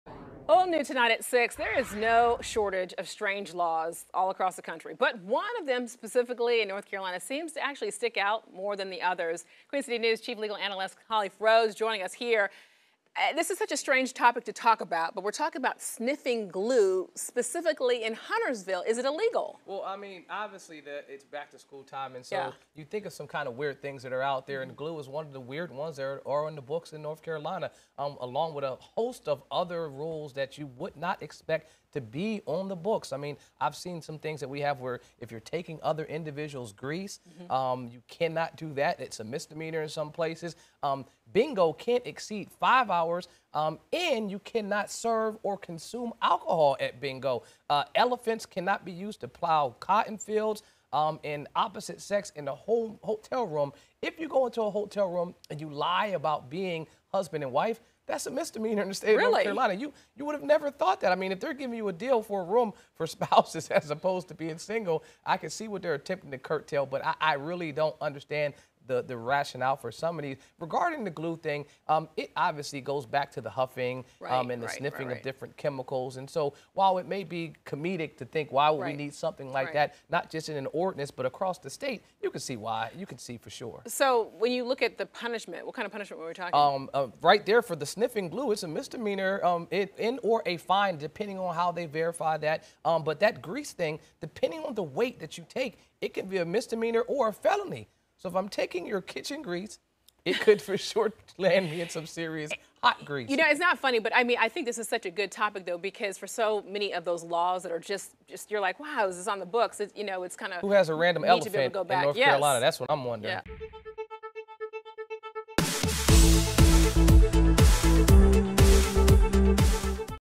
Check out weird laws in North Carolina
It's illegal to sniff glue in Huntersville.